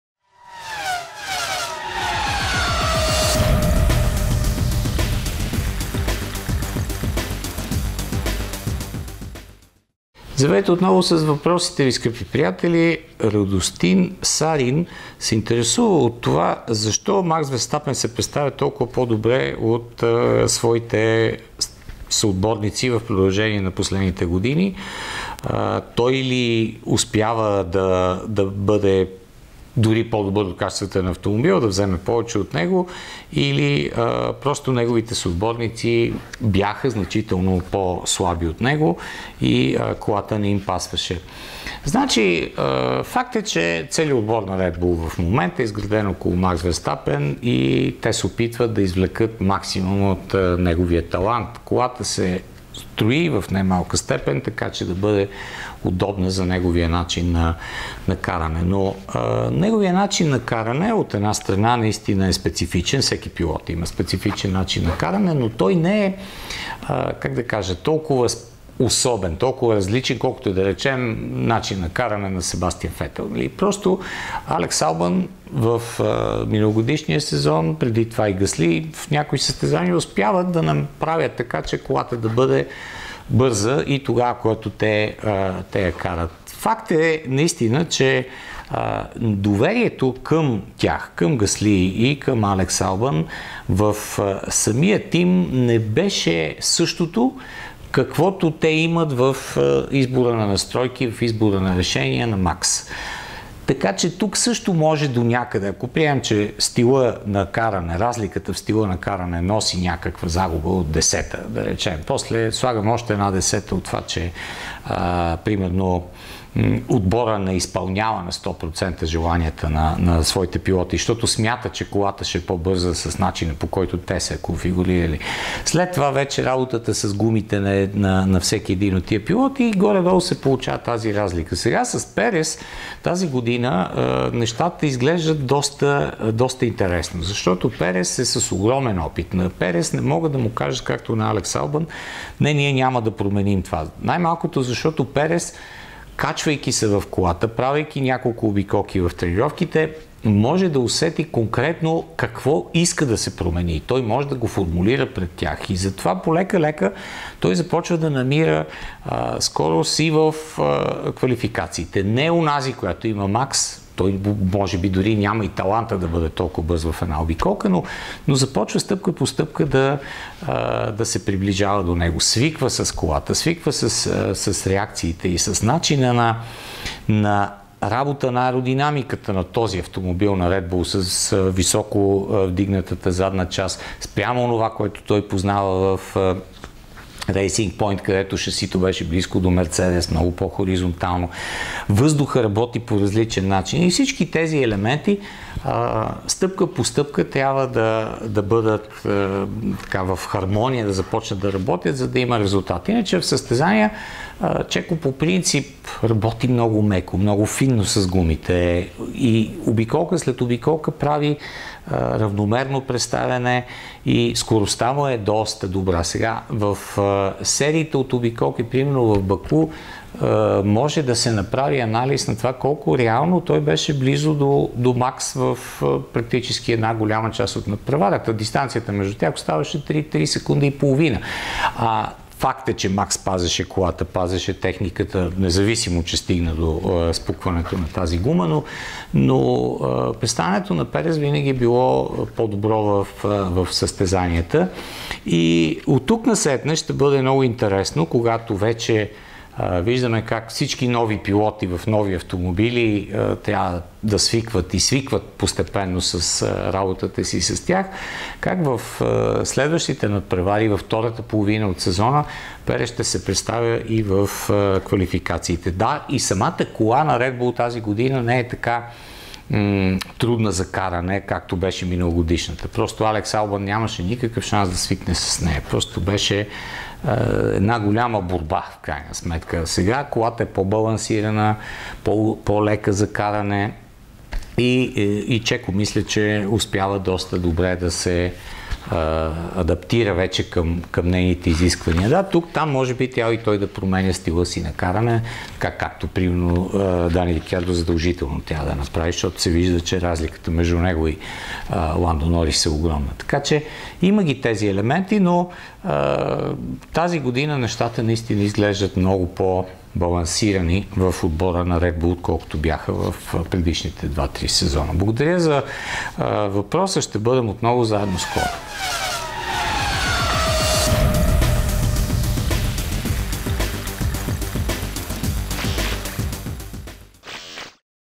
Представянето на Макс Верстапен
Моят въпрос е свързан със представянето на Макс Верстапен. От няколко години виждаме, как неговите съотборници изостават драстично спрямо неговото представяне. Толкова ли много Макс надскача потенциала на колата на Редбул или това се дължи на факта, че неговите съотборници са значително по слаби или колата не им пасва?